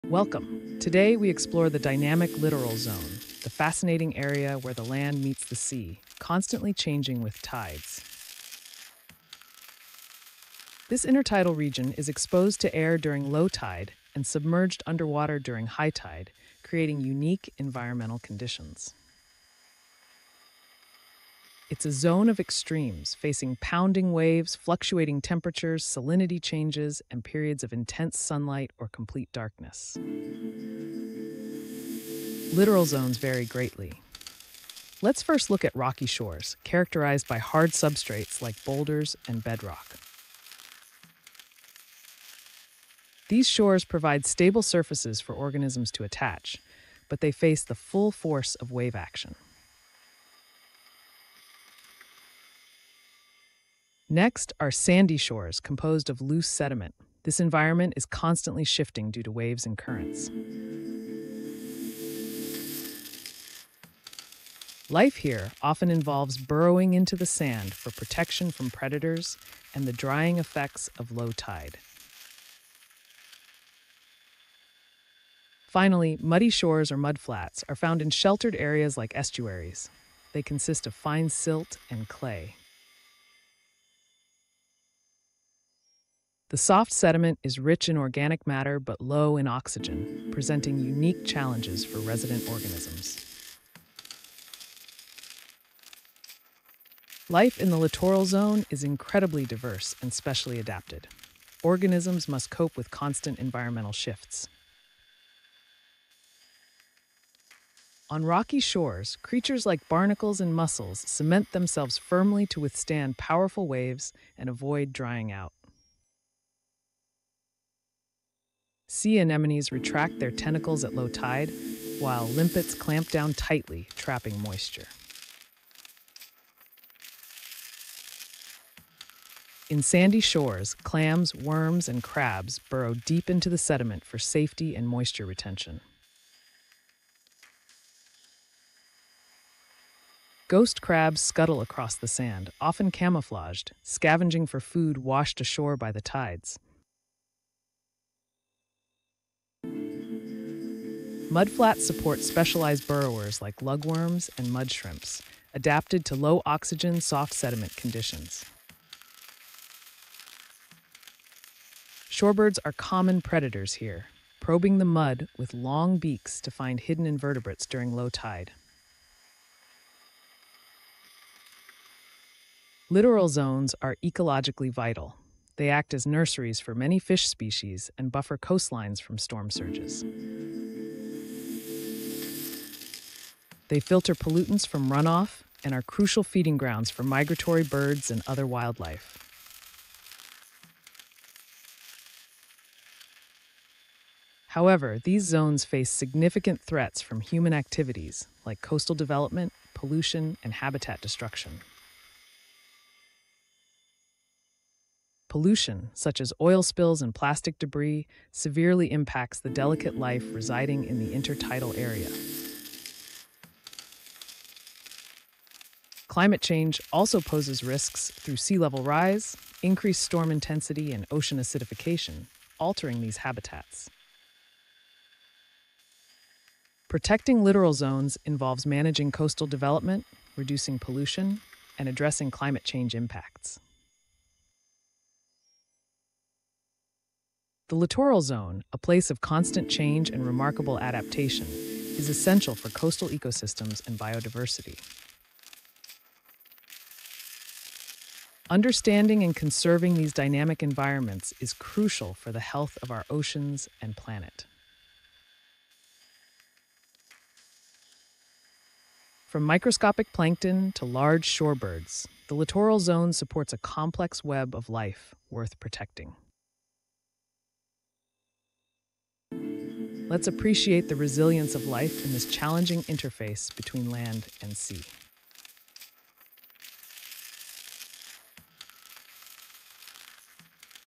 Littoral Wonderland: Secrets of the Shore Revealed 🌊
Dive deep into the mesmerizing world where the land meets the sea! Join us on a whimsical journey through the littoral zone—a place packed with surprises, from scuttling crabs to sneaky shorebirds! 🦀🐦

Discover what makes this zone just as epic as finding a message in a bottle. Get ready to be swept off your feet by the amazing adaptability of life on the edge of the tides, where waves play tag with the sand! 🌊⏳

This isn't just a geography lesson—it's an adventure with a splash of fun facts and a dash of humor! So, grab your sunscreen and flip-flops, and let's embark on this tide-turning exploration. And remember, the tides wait for no one!